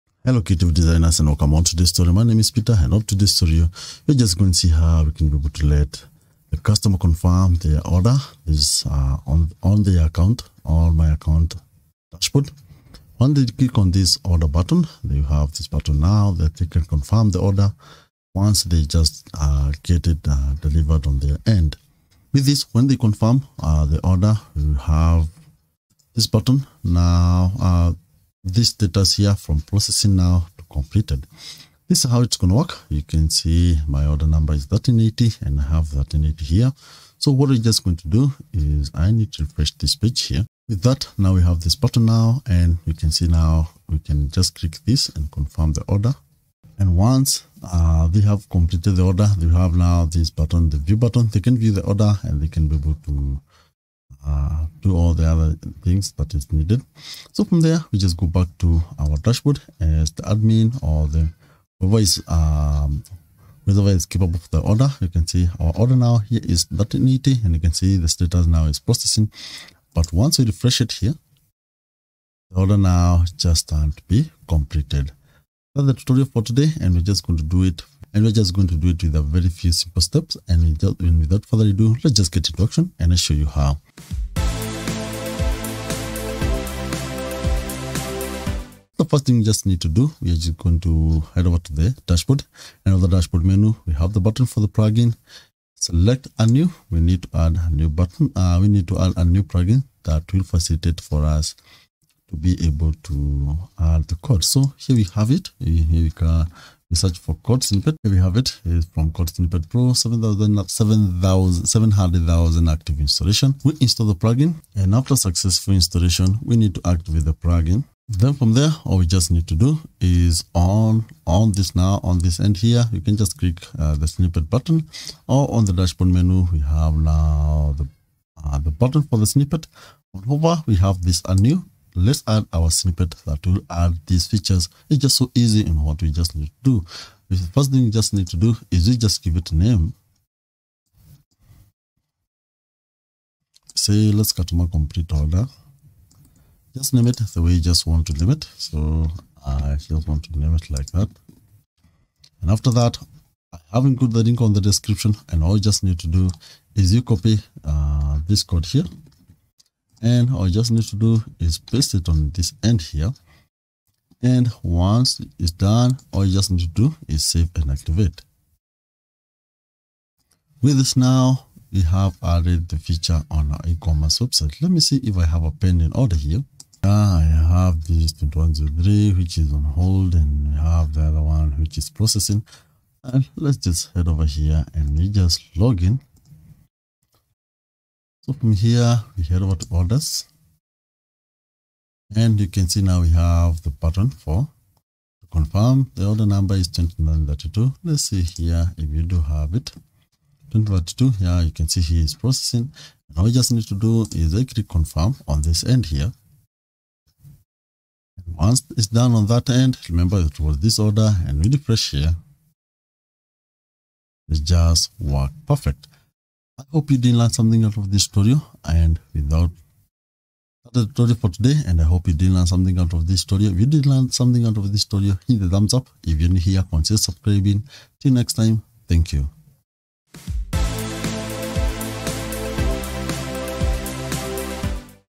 WooCommerce: Let Customers Complete a Processing Order.
In this woocommerce tutorial, we are going to show you how to set up a customer confirm order button on my account. This button will allow your customers to confirm their order from processing to complete Once the order has been delivered. This video will go over the steps needed to create the button and show you how to use it.